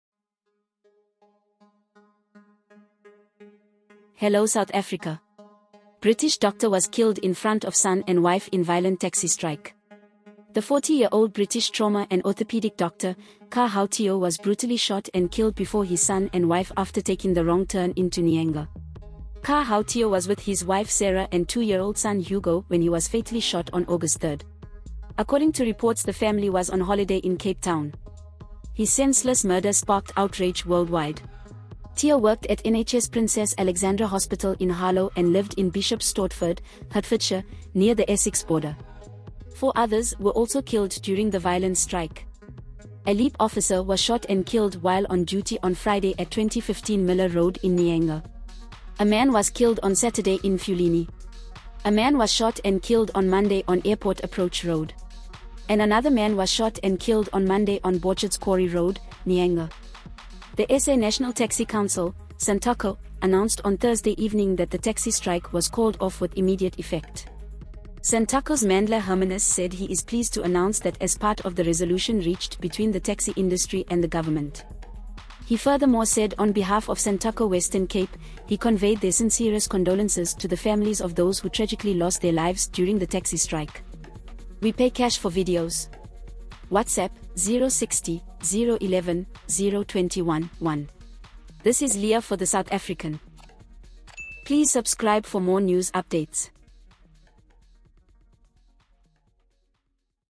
British doctor was killed in front of son and wife in violent taxi strike | NEWS IN A MINUTE
The 40-year-old British trauma and orthopaedic doctor, Kar Hao Teoh was brutally shot and killed before his son and wife after taking the wrong turn into Nyanga. Kar Hao Teoh was with his wife Sara and two-year-old son Hugo when he was fatally shot on August 3. According to reports the family was on holiday in Cape Town.

VIDEOS: Pixabay